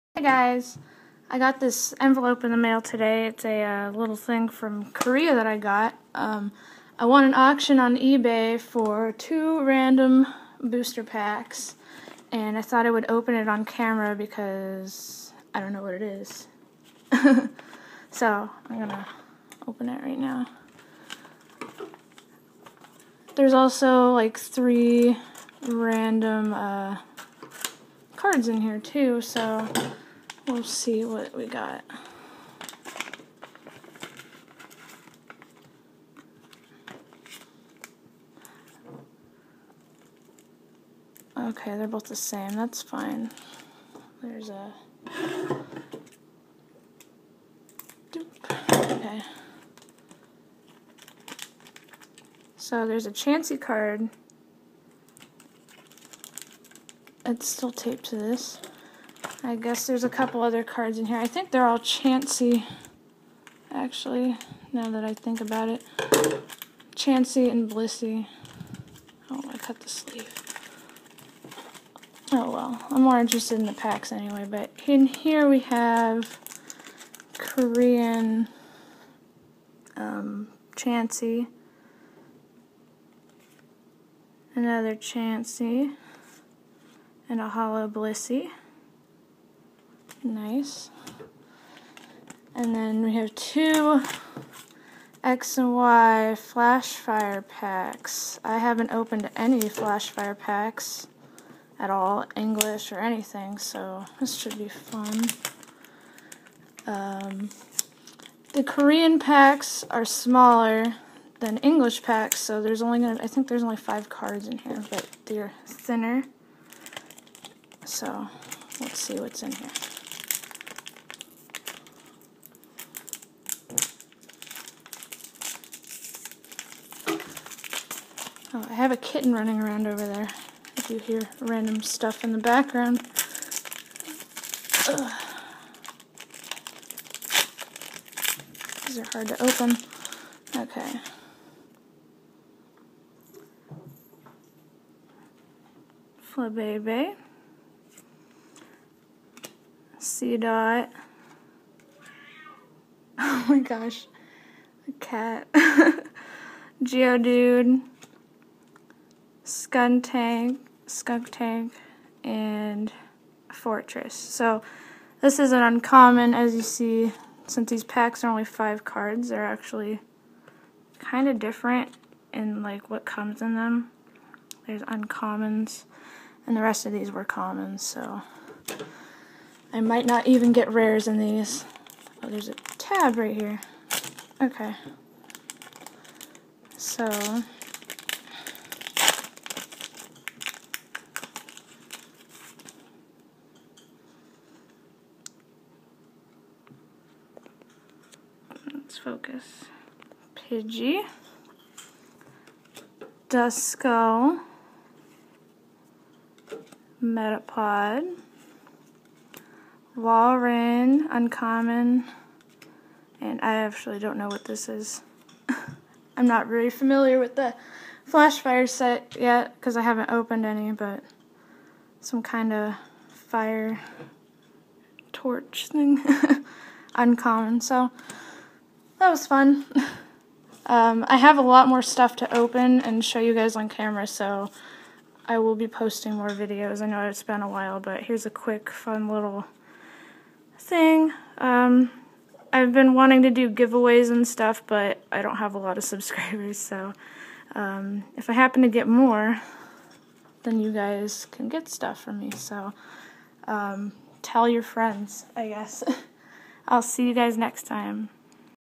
Opening Two Random Korean Booster Packs!
Just a quick video because it's been a while! This is from eBay. Three loose single cards along with two random booster packs! The two packs ended up being Wild Blaze (XY Flashfire) , which I have not opened at all (in English either). Nice surprise!